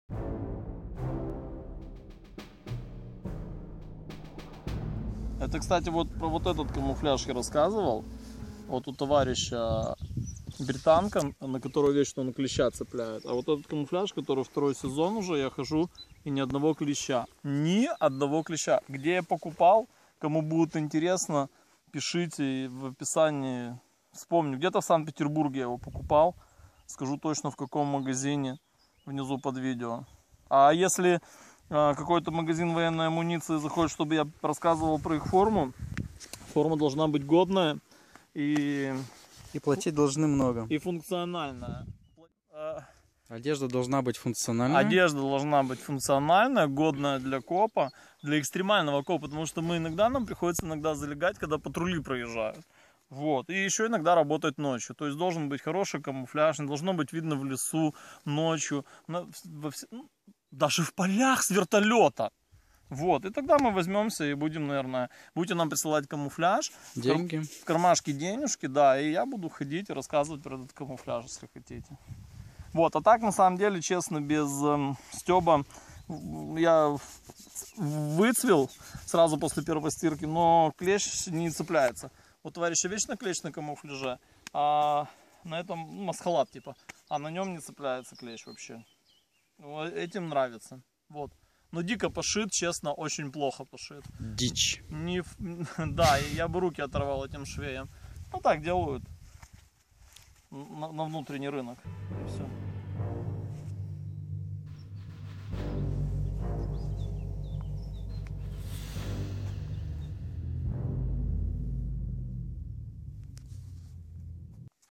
Камуфляж который защитит от клеща
Дорогие и любимые милитаристы представляю Вам купленный где то в СПб камок который я юзаю уже больше двух сезонов, камуфляж это или масс костюм не важно !
Для начала я не оценил возможности данного лесного костюма и он мне не понравился, минусы и плюсы данного костюма кинулись в глаза:
МИНУСЫ камуфляжа
- пошит из рук вон плохо согласно антотомическим показателям
- после 5 стирок камуфляж сразу побледнел (в каких то моментах это и ПЛЮС) особенно когда ведёшь скрытое перемещение по лесу !
ПЛЮСЫ лесного костюма
- клещ не может зацепиться на данную ткань
Итак плюс который я не сразу заметил при использовании данного костюма мне открылся не сразу. Ношу его второй год естественно 2 сезона это лето и тёплые времена года, каждые выходные часов 8 минимум в день, после копа естественно стирка и сушка на солнце.
Плюс этого костюма я заметил на второй год его использования, я попросту перестал на себе снимать клеща. Клещ у нас водиться всё лето осень и весну, был период и зима была тёплая и зимой мы клещей цепляли, но чем больше на человеке одежды, тем легче клещу зацепиться за человека, а вот летом когда я стал использовать данный костюм, который мне рисунком камуфляжа напоминает “орех” немецкого я стал приезжать домой и странным образом на мне не было ни одного клеща.
При сравнение с костюмом моего напарника а у него “Британка” то в первые 2 часа копа он уже мне в лесу хвастался, тем, что на нём уже сидел как минимум 1 клещ, и тогда мы начали проверять в чём причина, и при детальном рассмотрении структуры ткани его и моего камуфляжа мы обратили, что мой комок “лысый”а структура его “Британки” попросту ворсистая, вот за этот самый ворс на его камуфляж и цеплялся клещ, который потом в процессе копа благополучно переходил на хозяина костюма !
При поддержке сайта по продаже военных трофеев времён второй мировой войны schweik.ru